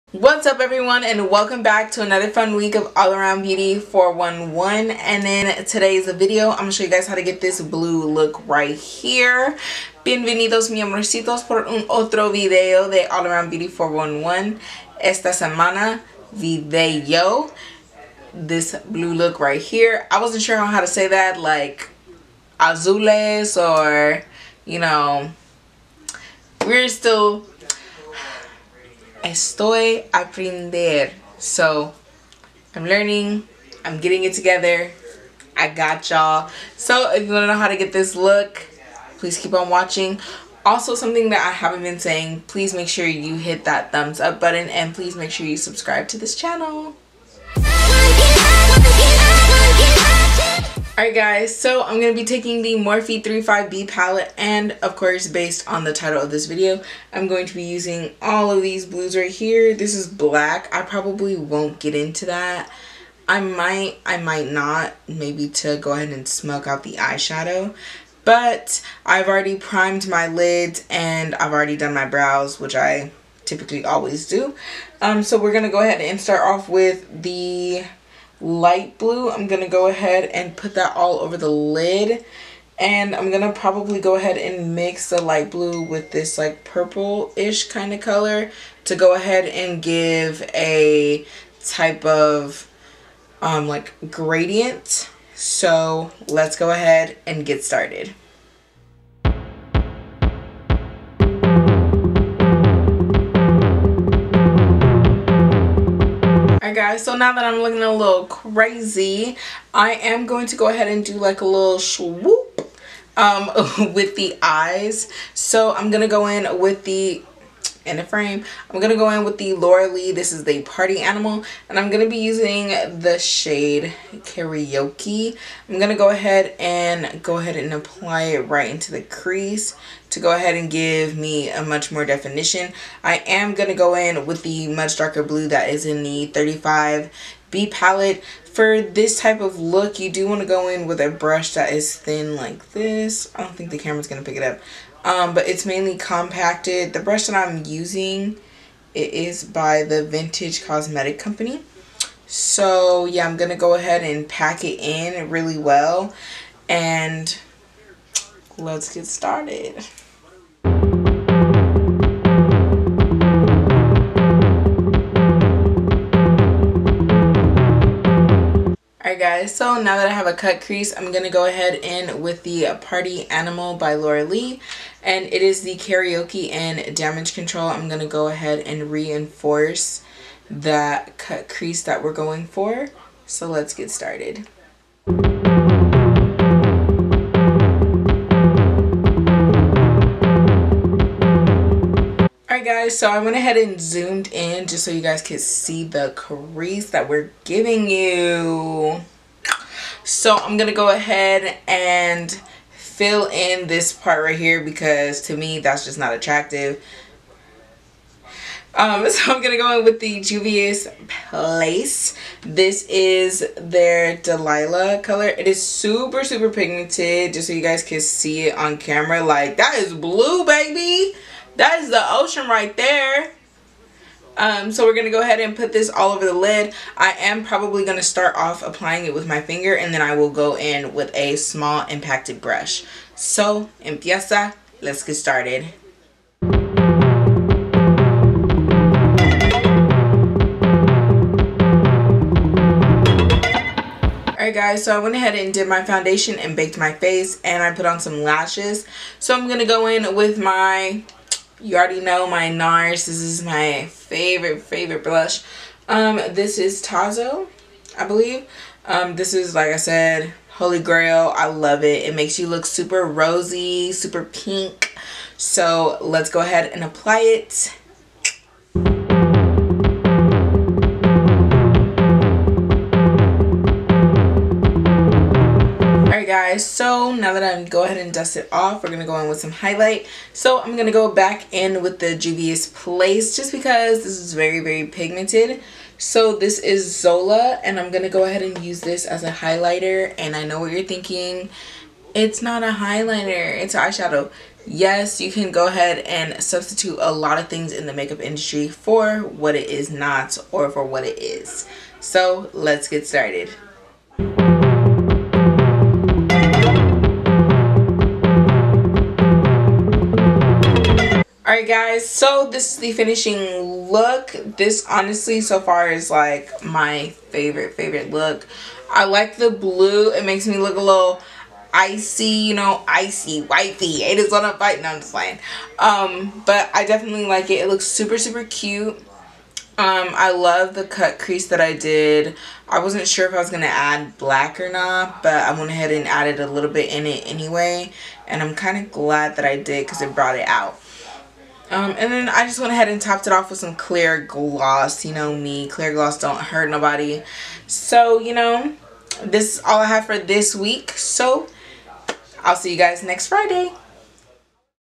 I'm Feeling Blue | Makeup Tutorial
Hey loves, this blue look is giving me Austin Powers vibes...yes the one with Beyonce in it! Also if you decide to give this a try tag me on Instagram or FaceBook.

Products Used in Video:

 Face Primer  by Omalewa Cosmetics

Creamy Concealer (Amande) by NARS

Concealer by Concealer Collection

Blush (TAOS) by NARS

Eyeshadow Primer by Sigma Beauty

Eyeshadow Palette (35B) by Morphe

Eyeshadow Palette (Party Animal) by Laura Lee

Mascara (Man Eater) by Tarte Cosmetics

Brow Contour by Benefit Cosmetics

Setting Powder (Almond/Chestnut) by Beauty Bakerie

Lipgloss (clear) by ELF

All products and company names referenced in this video are trademarks of their respective holders.